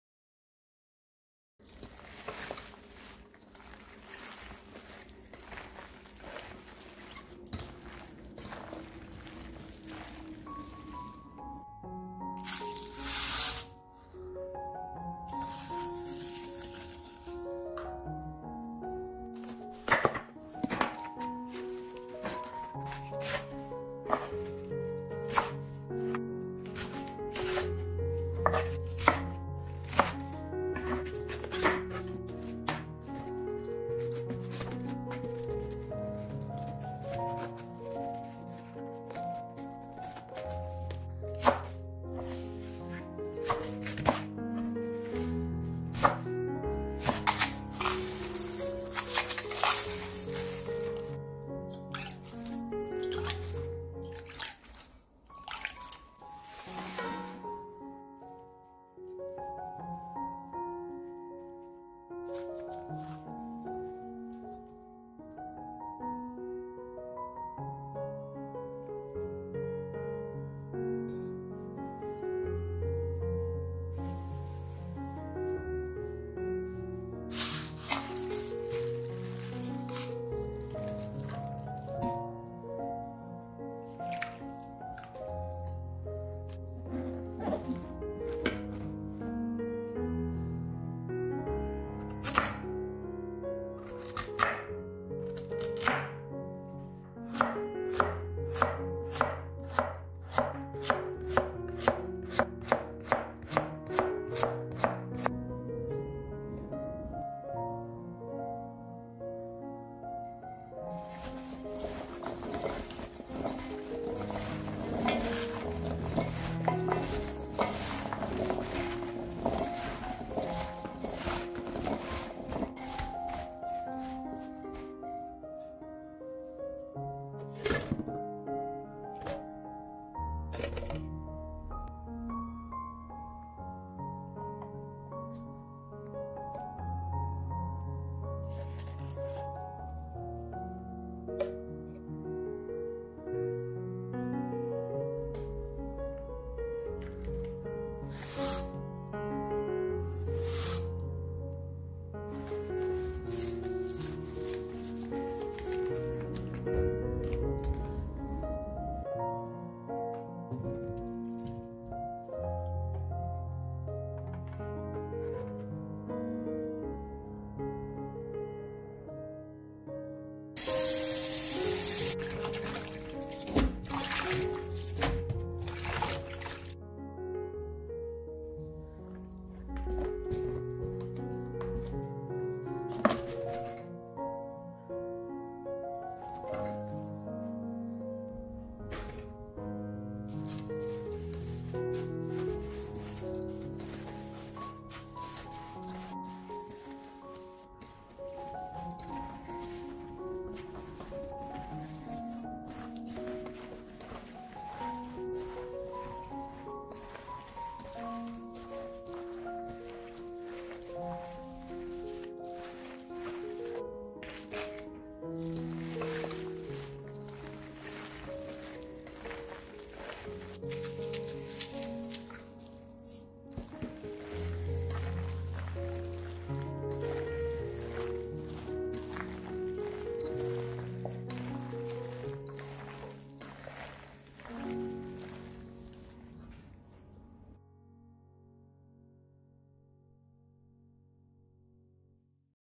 맛김치 맛있게 담그는 법 김치담그는법 김
맛김치 맛있게 담그기
알배추 무김치

알배추 2 무1개
쪽파 1줌
고추가루 2컵반
천일염 1컵 반
꽃소금 1스푼
통깨2
배 양파 반개씩
마늘 1줌
생강청 1
새우젓 2
멸치액젓 60ml
매실청 30ml
찹쌀풀 150ml